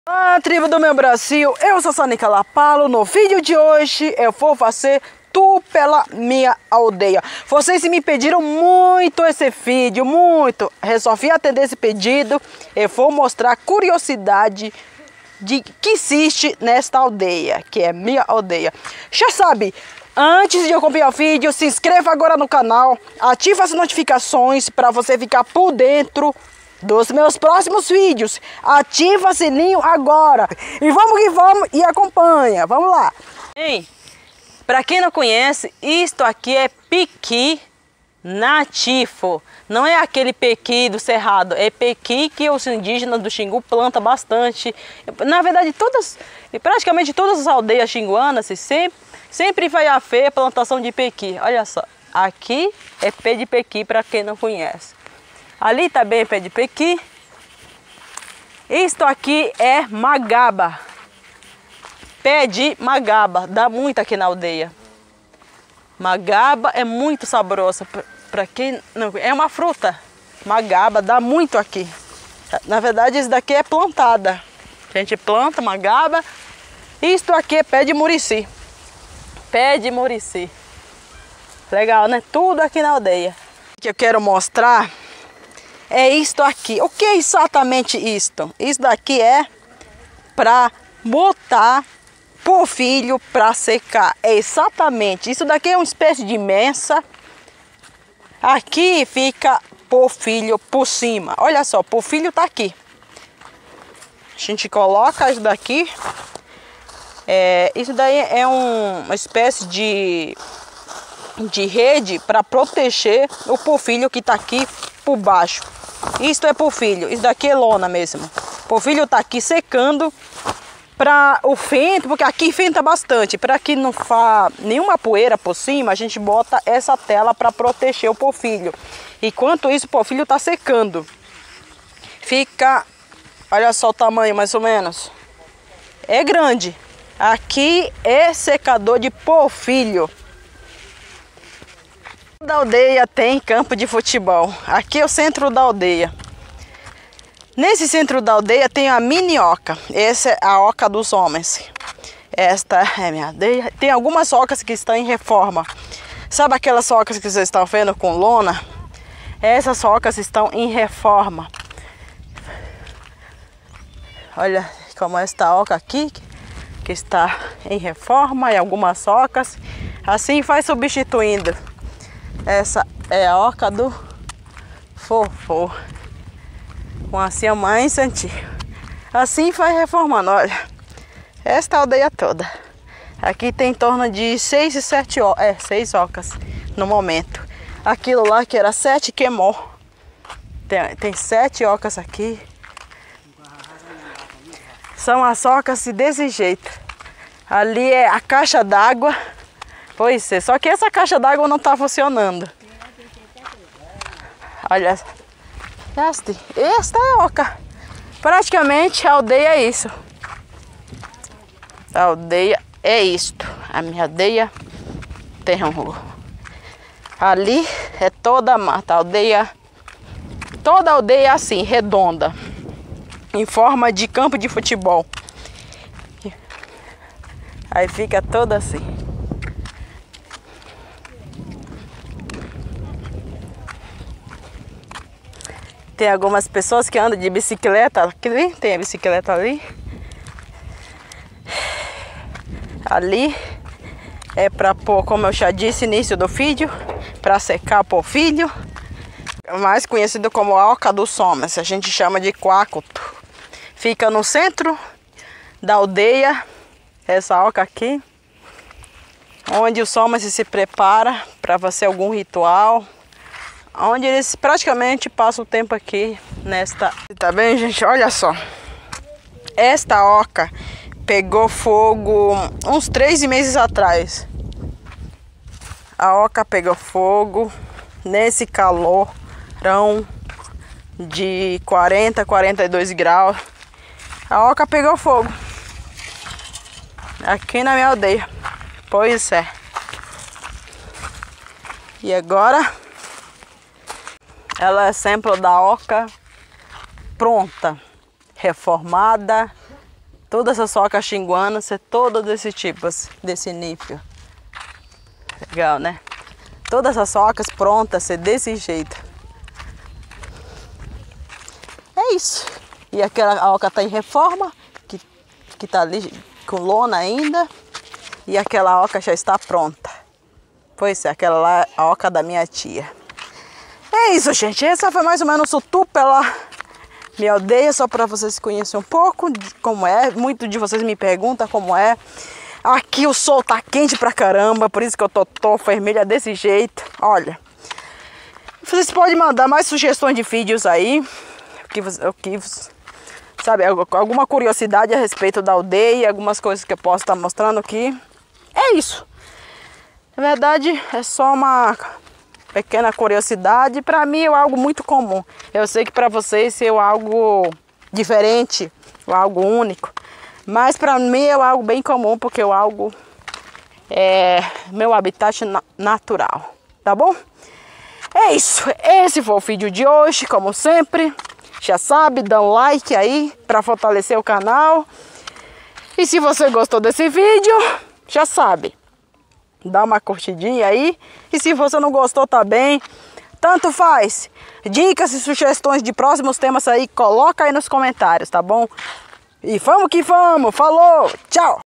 TOUR PELA MINHA ALDEIA
Neste vídeo eu mostro um pouco da minha aldeia Tehuhungu, Parque Indígena do Xingu.